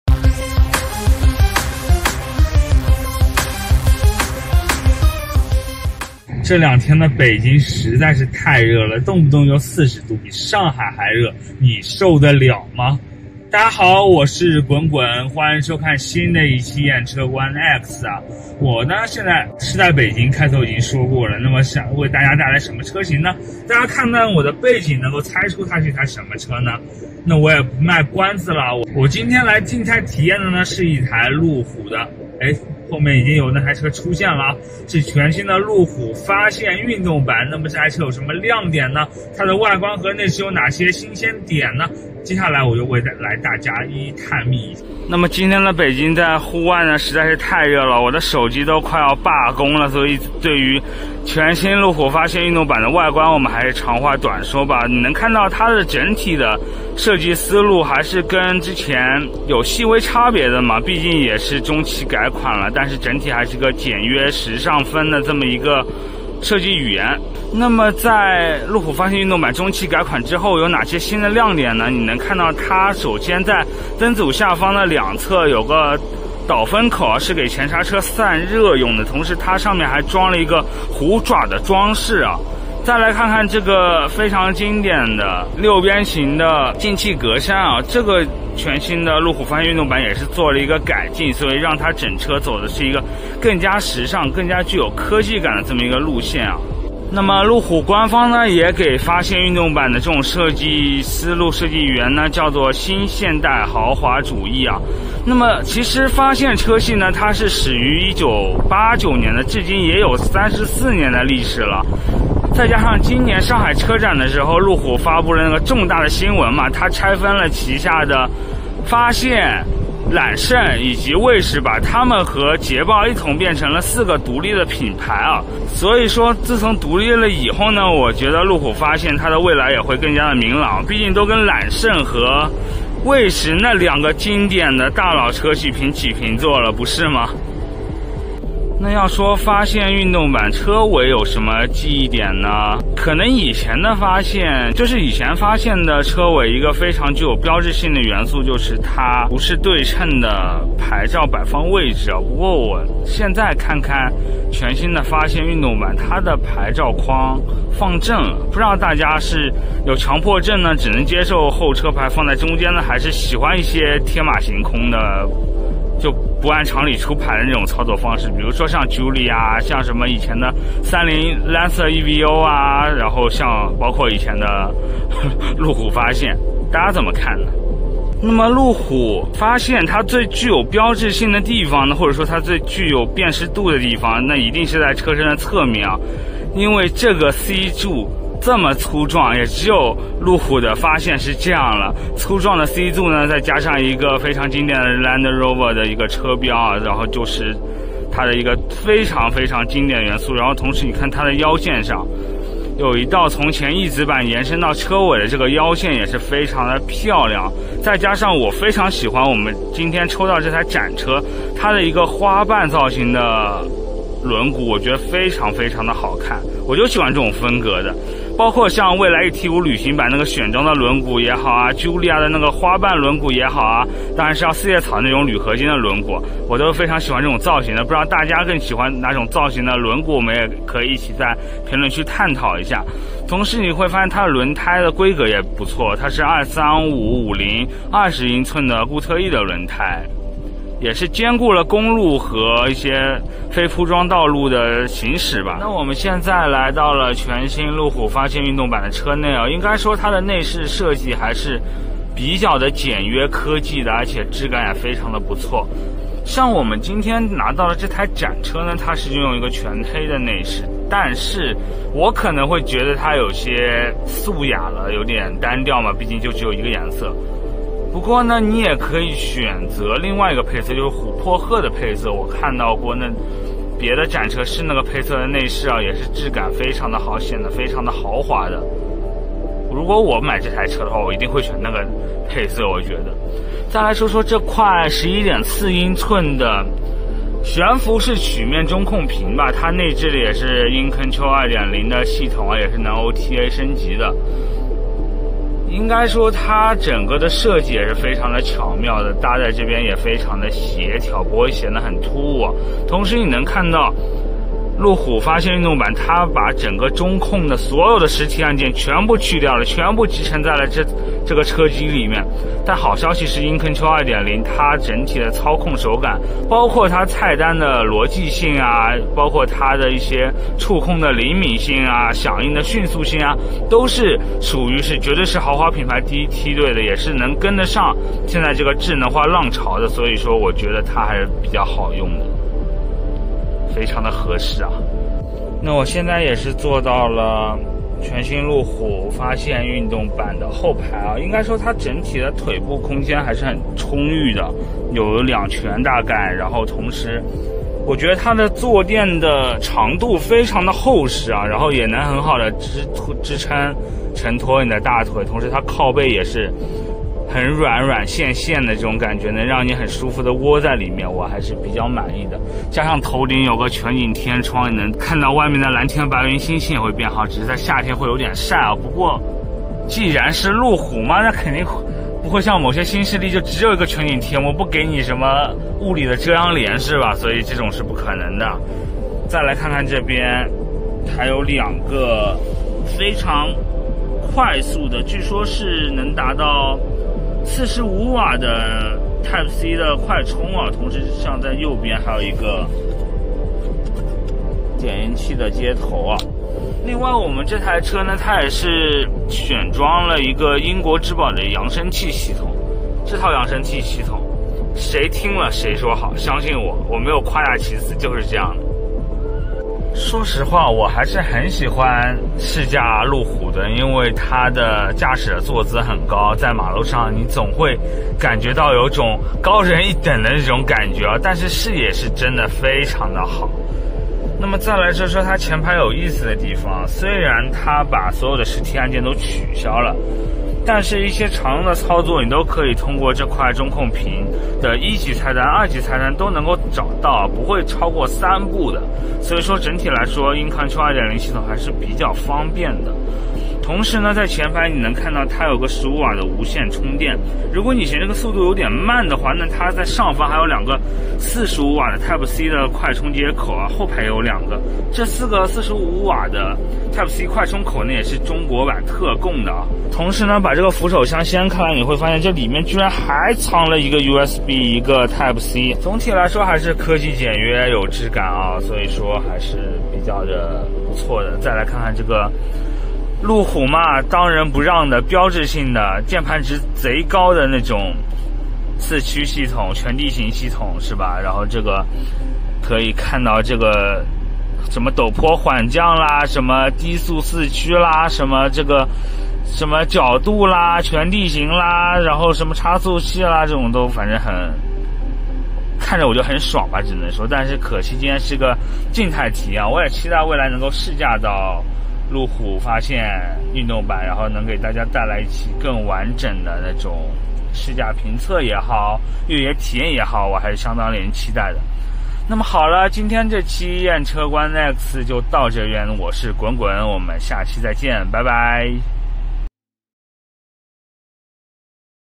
去探索，去发现！全新路虎发现运动版的无限可能
在今年的上海车展上，路虎发现与卫士、揽胜、捷豹一同成为了四大子品牌之一。让我们一同看看全方位进化升级的路虎发现运动版会为我们带来哪些惊喜吧！毕竟它的很多设计很多配置都是为国内市场量身打造的哦！